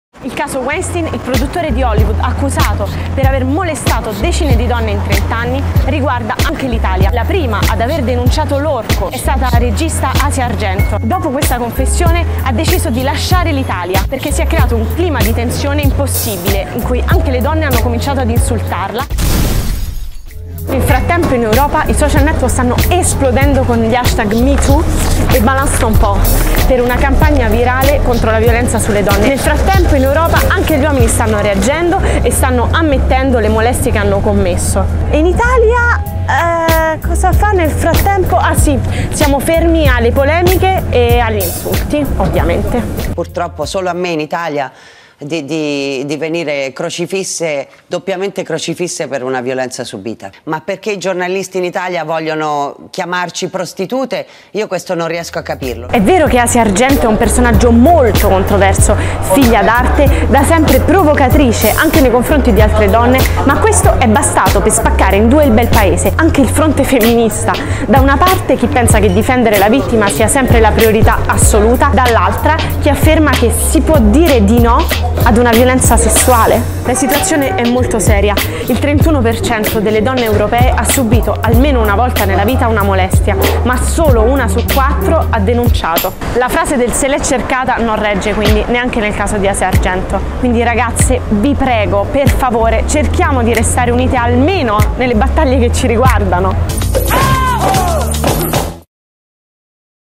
Who cares? - Italy: Women divided on Weinstein case
In bashing actress and director Asia Argento after she accused Weinstein of harassing her, Italy is showing its deep misogynistic nature. Insults and victim-blaming have pushed Asia Argento to leave her homeland. Who cares? We do, once again.